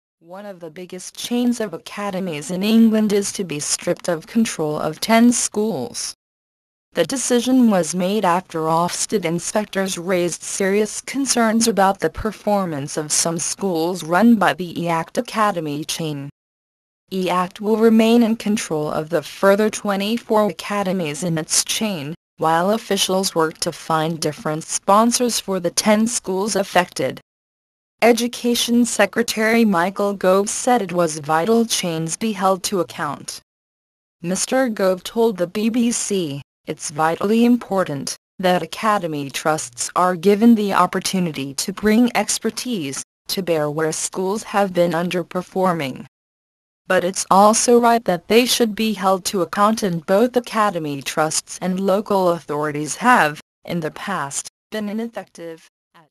E-Act chain loses control of 10 academy schools..
One of the biggest chains of academies in England is to be stripped of control of 10 schools.

The decision was made after Ofsted inspectors raised serious concerns about the performance of some schools run by the E-Act academy chain.

E-Act will remain in control of the further 24 academies in its chain, while officials work to find different sponsors for the 10 schools affected.

Education Secretary Michael Gove said it was vital chains be held to account.

Mr Gove told the BBC It's vitally important that academy trusts are given the opportunity to bring expertise to bear where schools have been underperforming.

But it's also right that they should be held to account and both academy trusts and local authorities have, in the past, been ineffective, at times, in bringing about the rate of change and the speed of change required.

So where they have been less than effective, we've decided that it's appropriate that action be taken.

Most academy chains had been highly successful in bringing about improvement, he added.

But Chris Keates, leader of the Nasuwt teachers' union said This 'pass the parcel' strategy is no way to treat children, staff and schools or to support school improvement.